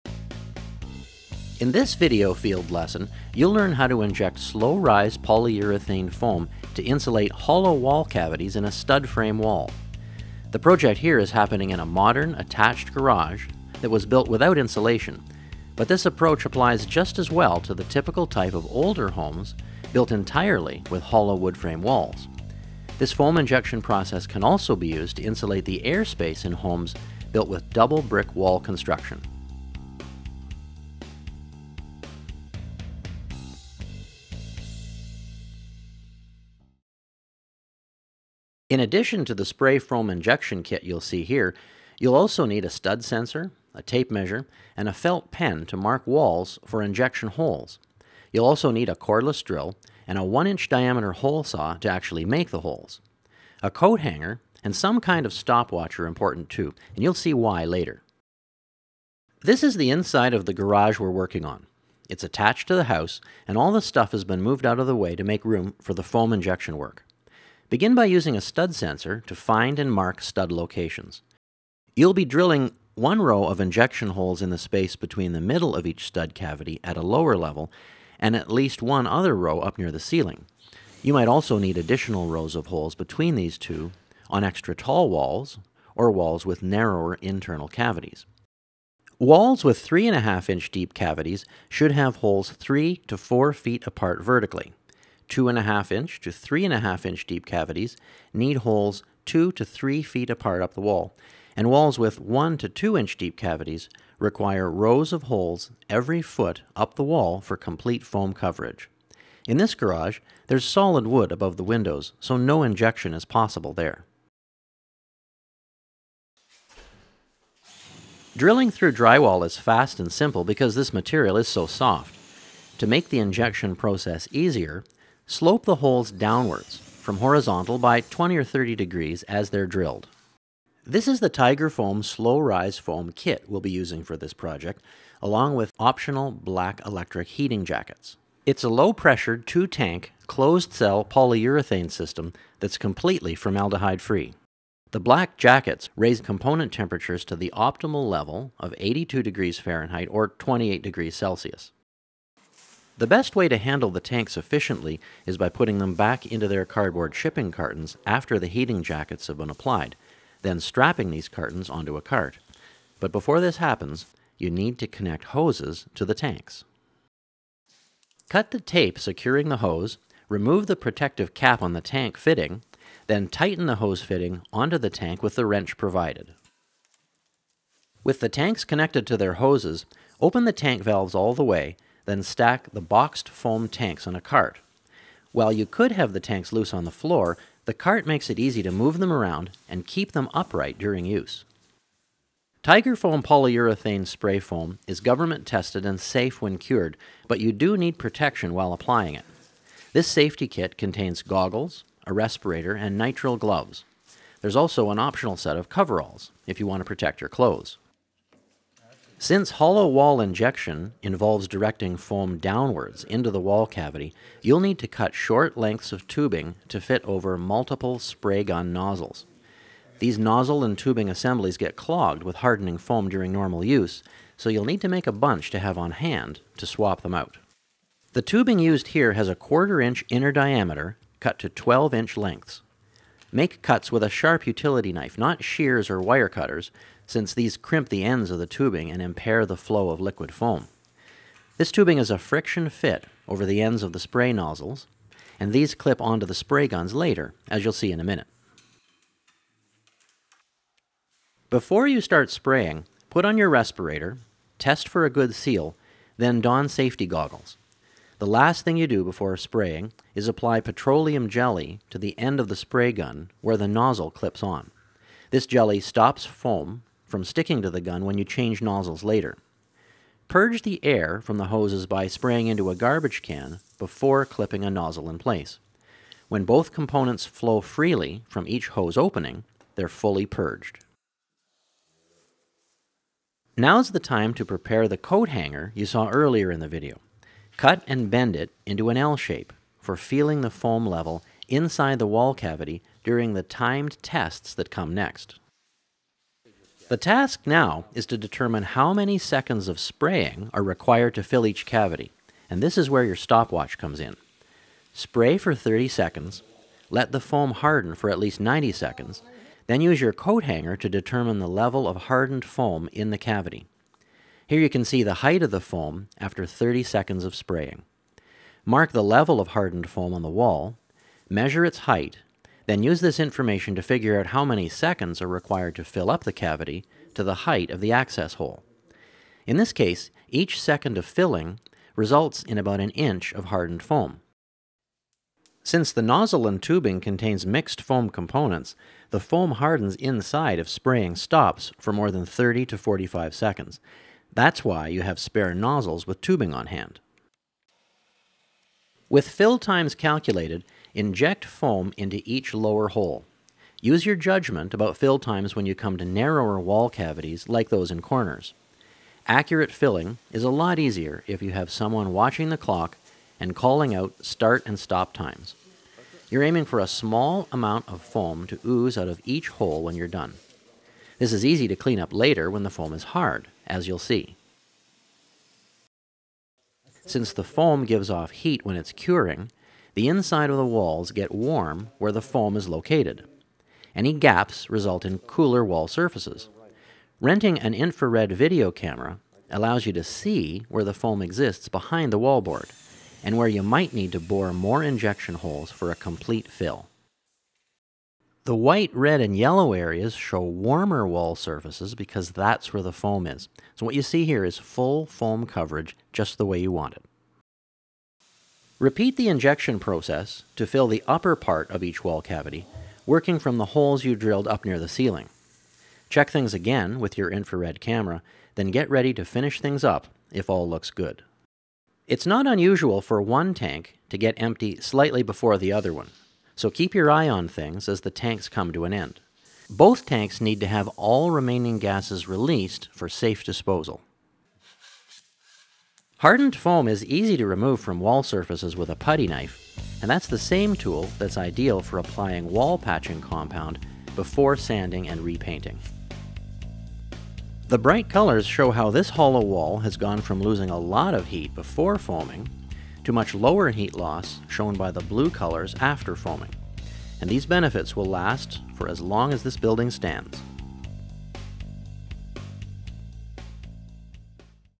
Upgrading Stud Wall Energy Performance With Spray Foam
Steve takes you on an 8-minute field tour showing how an uninsulated stud frame wall goes from very poor energy performance to very good performance.